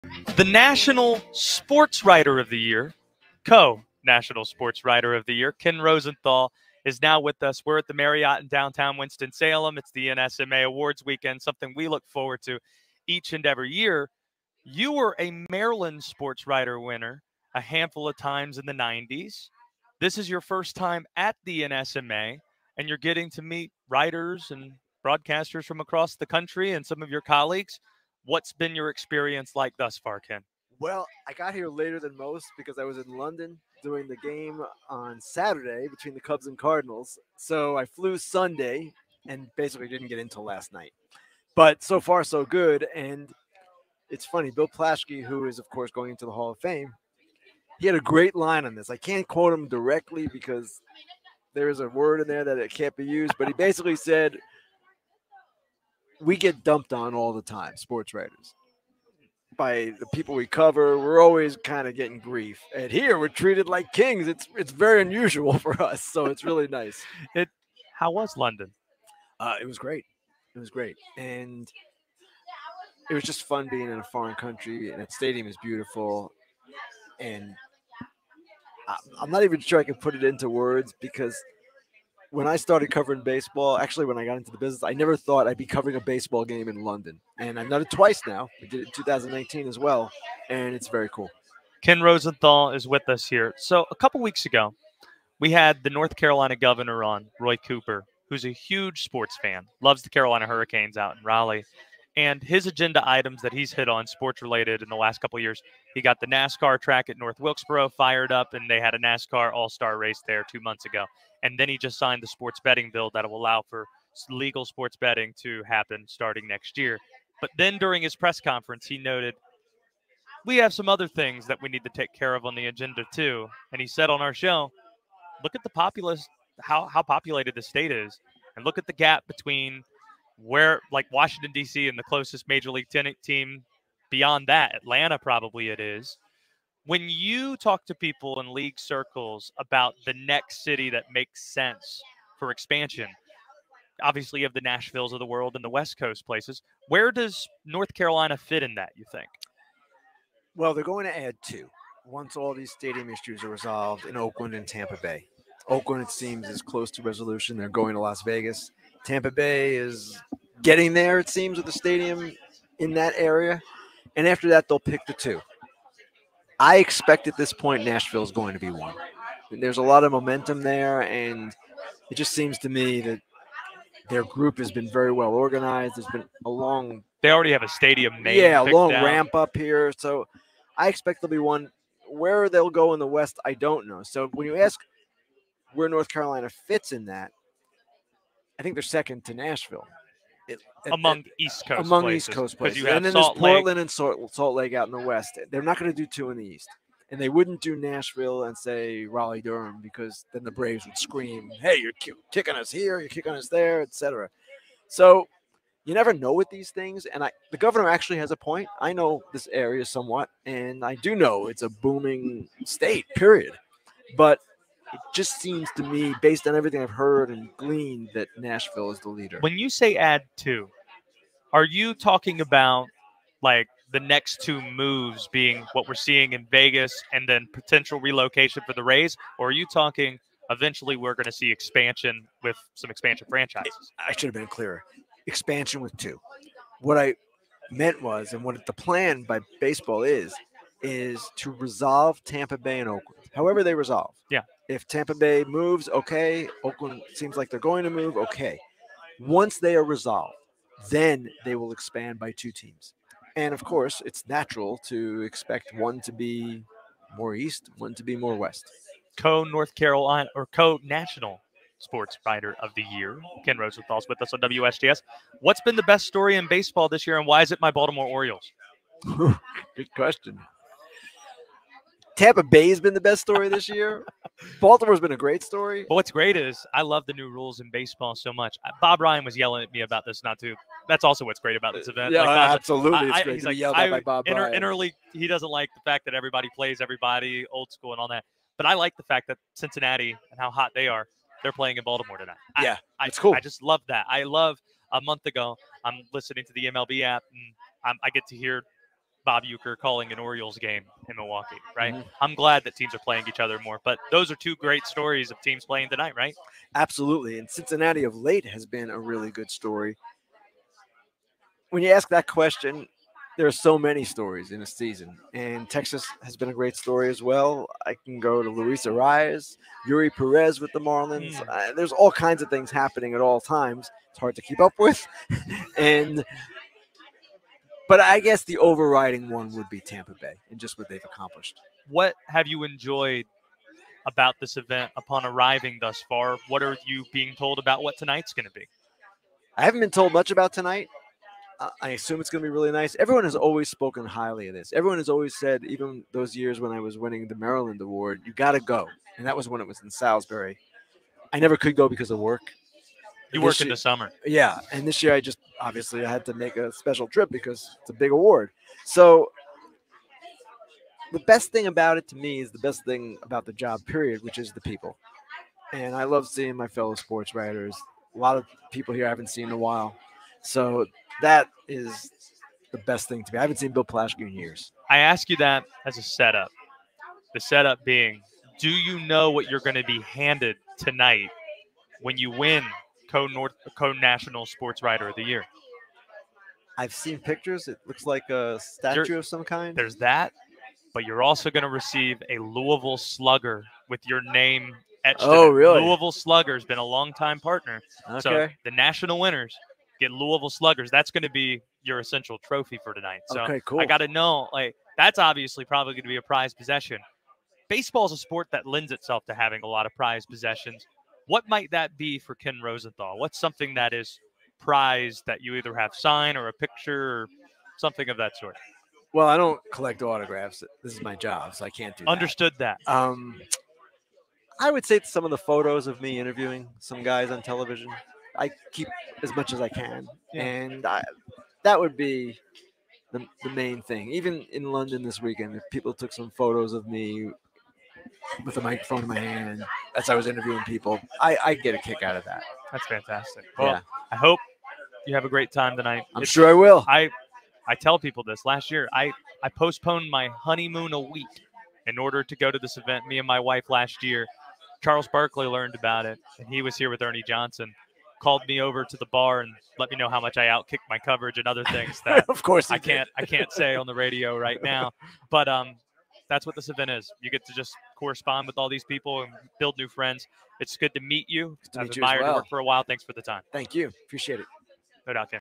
Ken Rosenthal: Nashville Is Best Position for MLB Franchise
Co-National Sportswriter of the Year, Ken Rosenthal, joins the show, live from the NSMA Awards, to discuss covering the MLB in London, where North Carolina fits in the MLB expansion conversation, and who the best story of the MLB season has been.